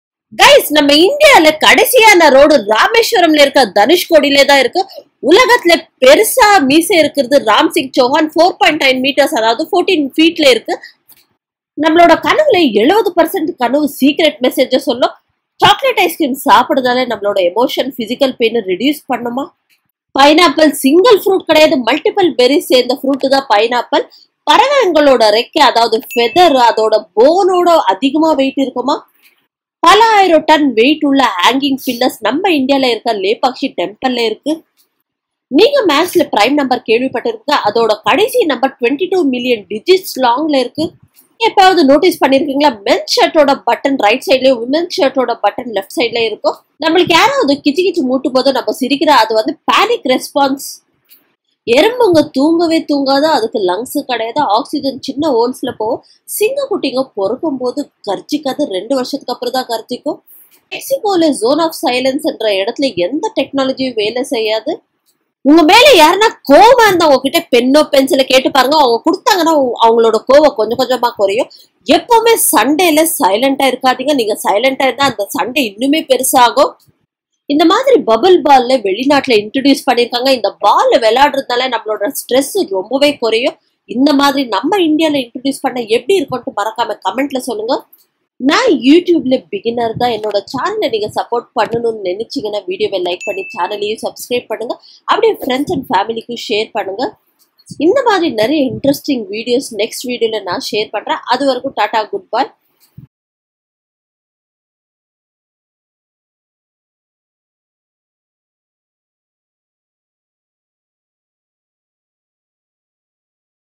Shocking Truths About Men's Vs Women's Shirts
Get ready to learn some interesting quick facts about men's and women's shirts in this video comparison! From fit to style, we'll explore the differences between men's and women's shirts.
In this Video you will be able to explore a lot of facts that blow your mind, like Men's Shirt Vs Women Shirts.